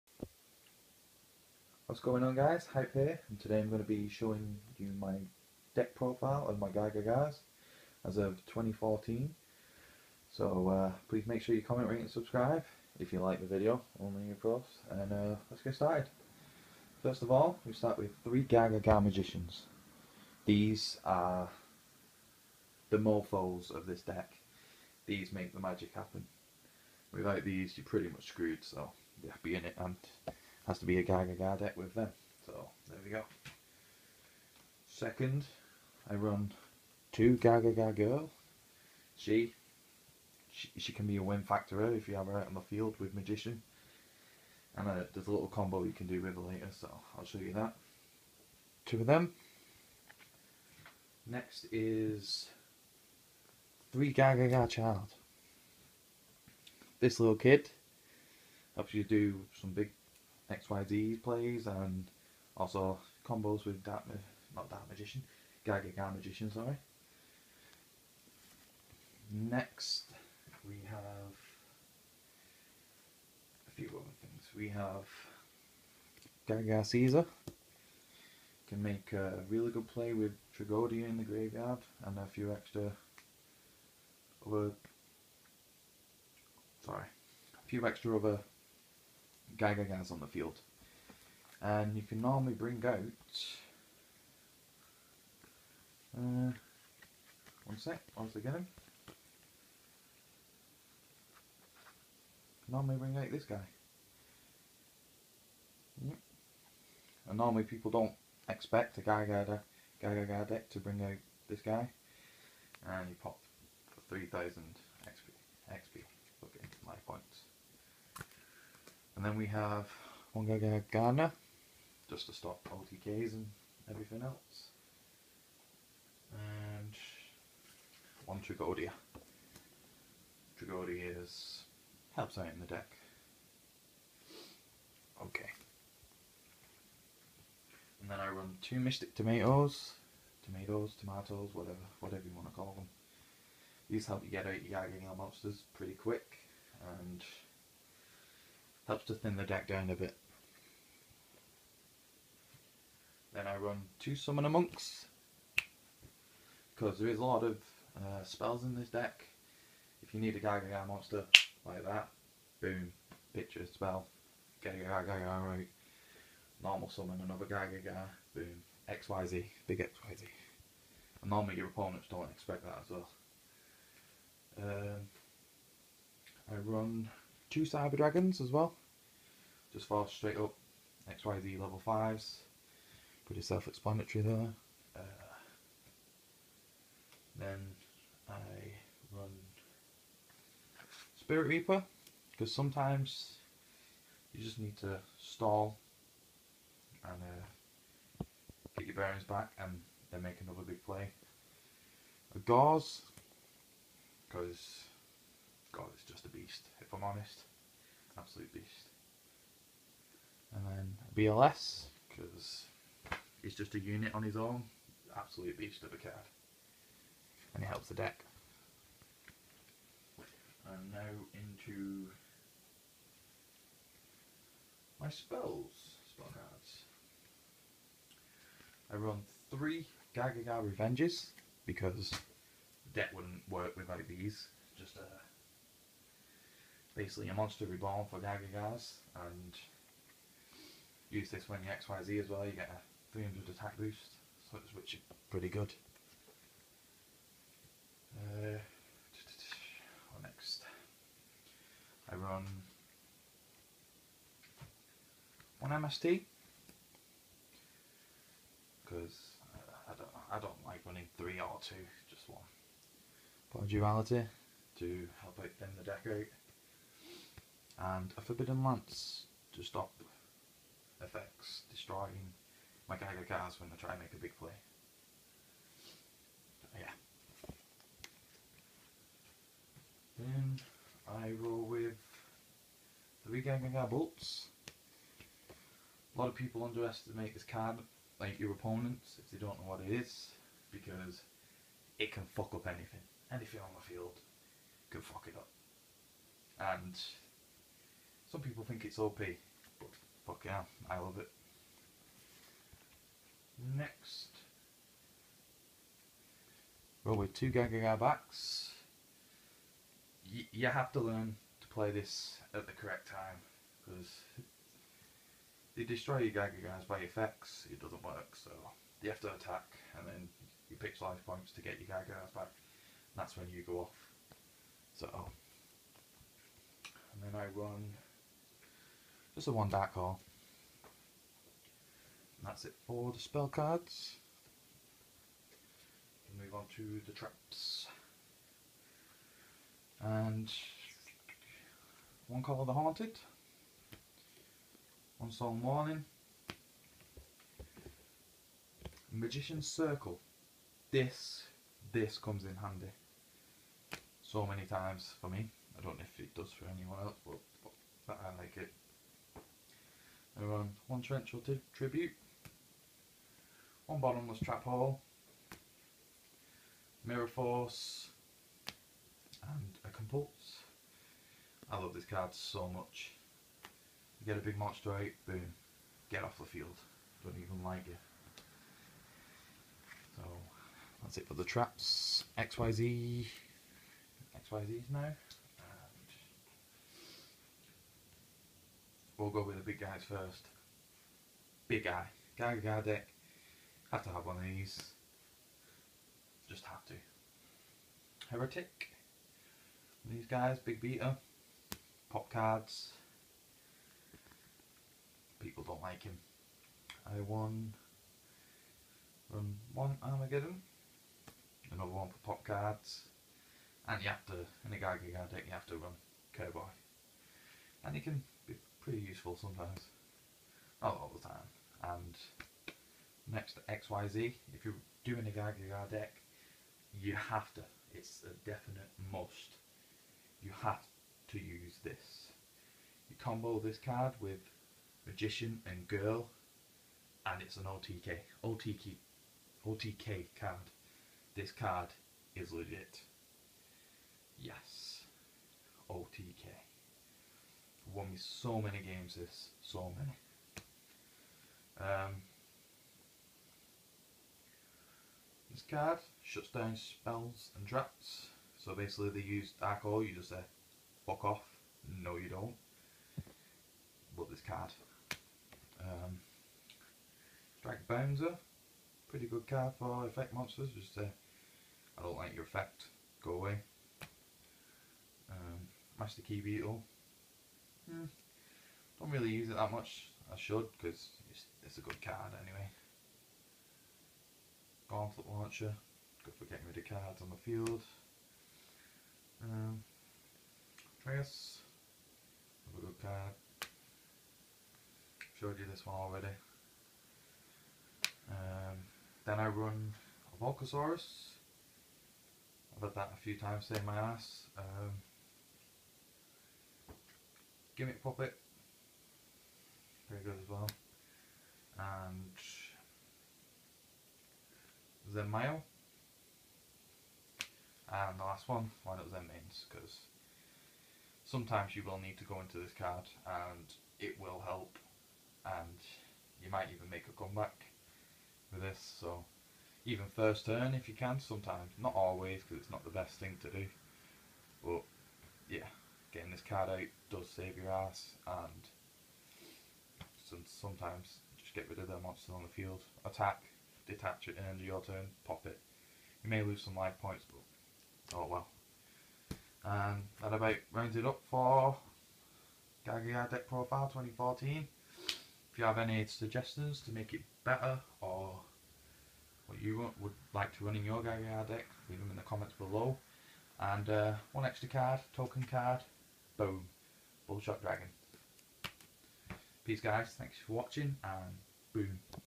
I'M back with GaGaGa Deck profile 2014
Hi all I'm back with a gagaga deck  profile 2014 this a fun deck and can put in work if you need it to. Let me no what you think and if you have any ideas